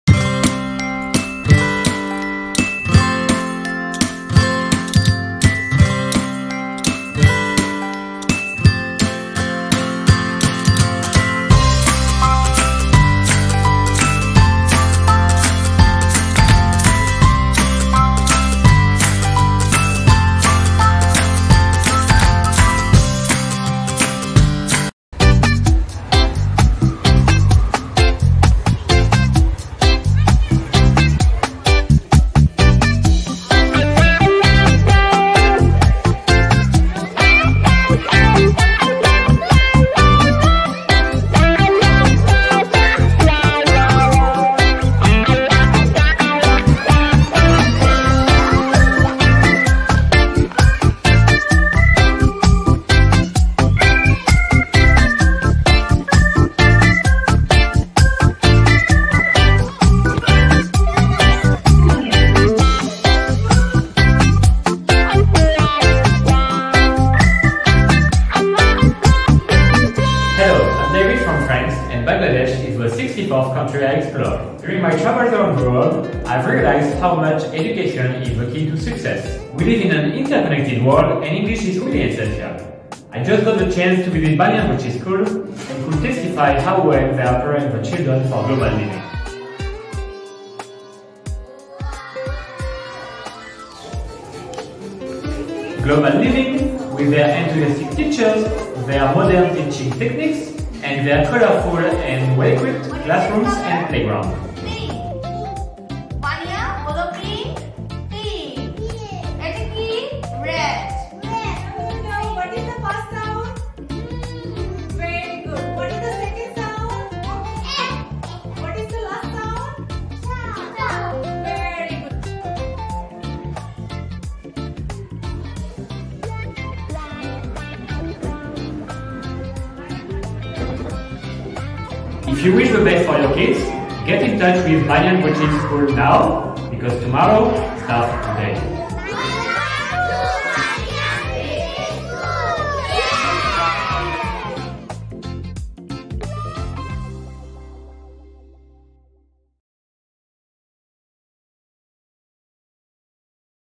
Best English Medium School In Sylhet | Banyan British School
Banyan British School is a British Curriculum, Cambridge Syllabus, English medium school located in Sylhet. We have classes from Play Group- A level.
The mission of Banyan British School is to provide each student with a diverse education in a safe, supportive environment where they can Learn, Play and Grow. We promote self-discipline, motivation, and excellence in learning. The school teachers and management team joins the parents and community to assist the students in developing skills to become independent and self-sufficient adults who will succeed and contribute responsibly in a global community.
Banyan British School is a private school providing a balanced and well-rounded education for Bangladeshi students.